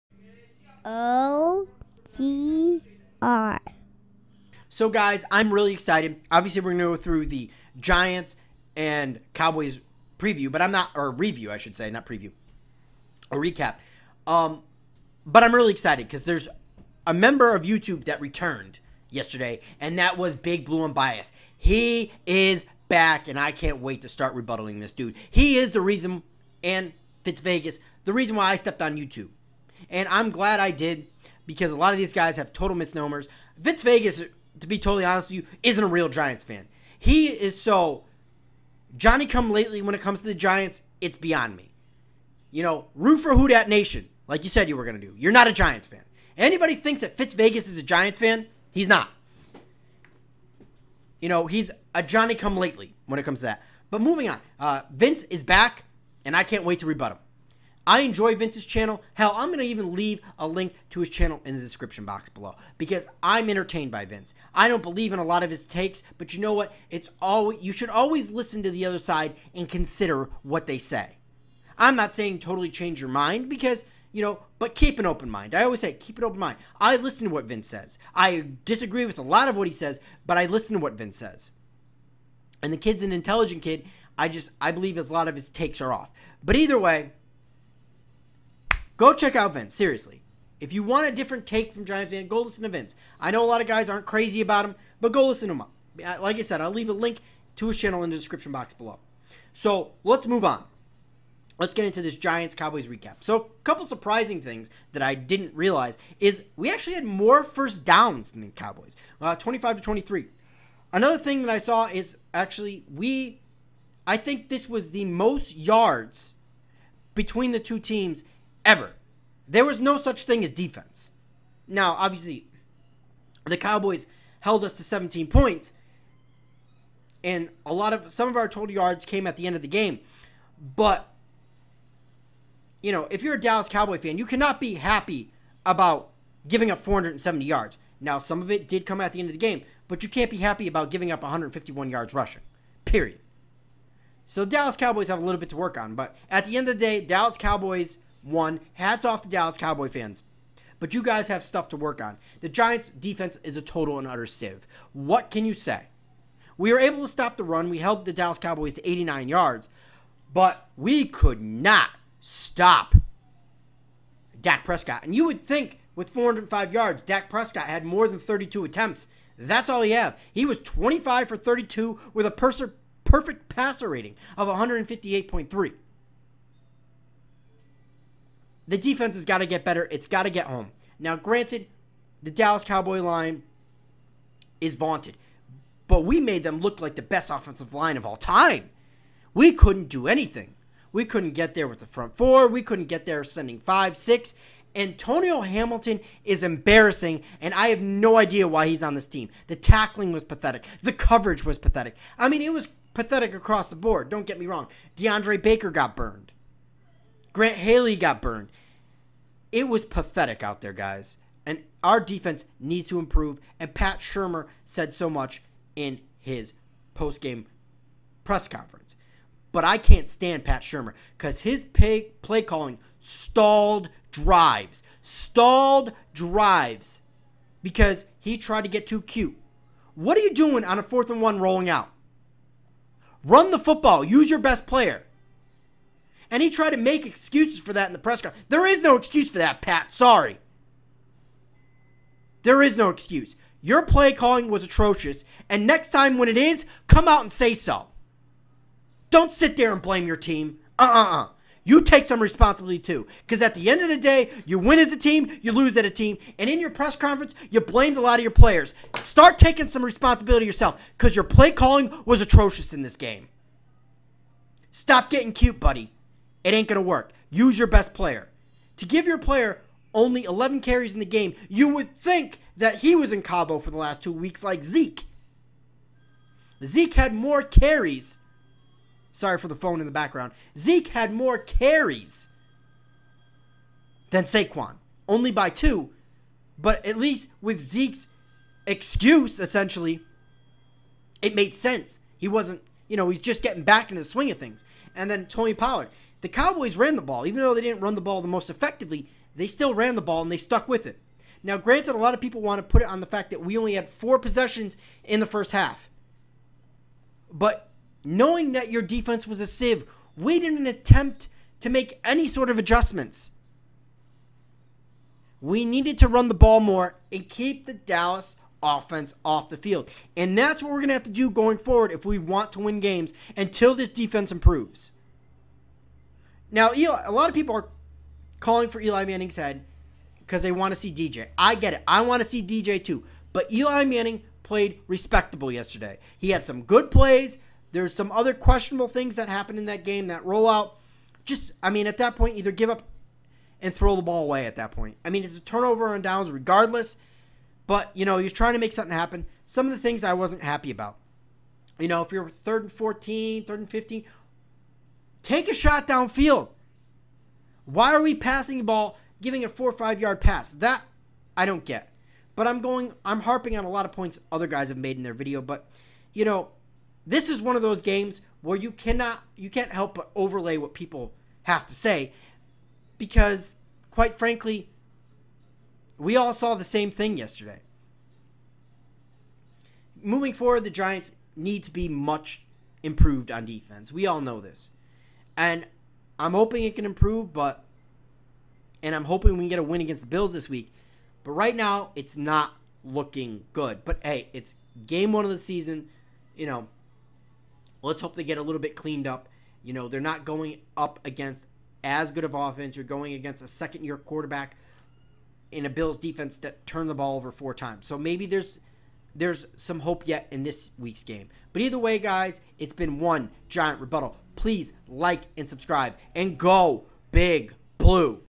Cowboy and Giants Recap/Reaction and BBU is Back!!!!!!
Defense needs to get better as stated above. Lets improve against these Bills. BBU is back I am excited I can't wait to rebuttal him.